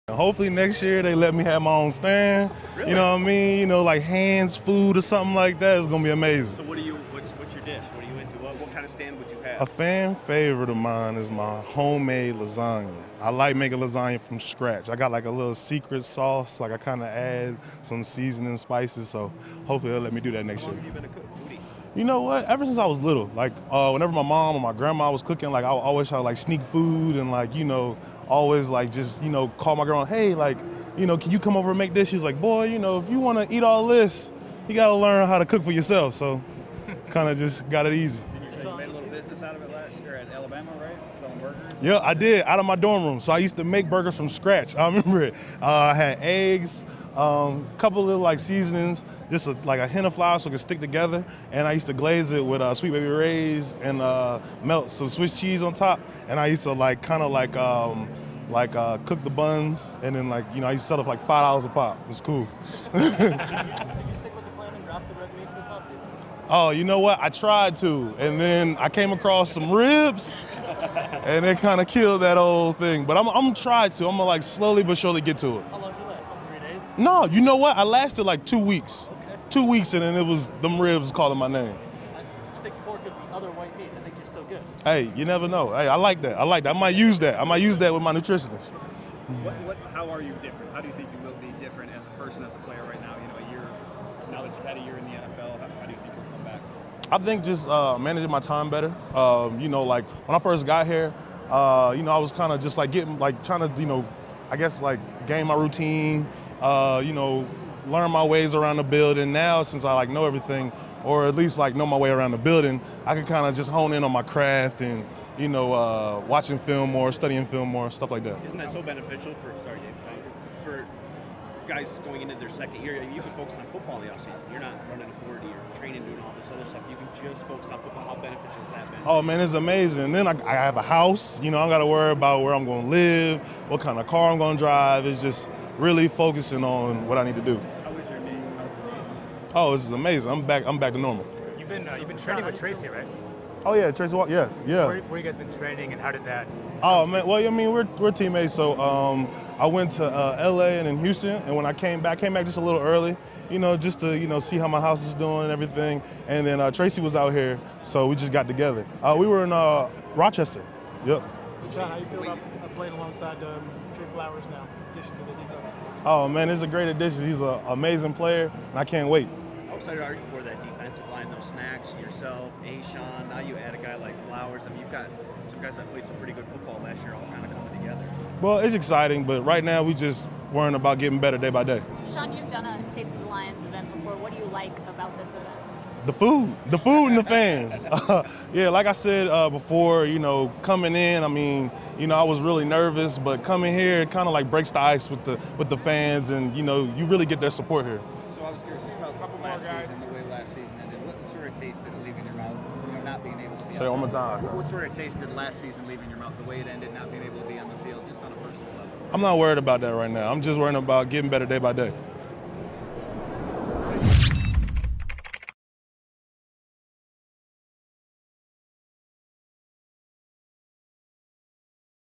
Da'Shawn Hand on his favorite recipes | Detroit Lions Sound Bites
DL Da'Shawn Hand speaks with the media at Taste of the Lions about his favorite recipes and his focus heading into his second year.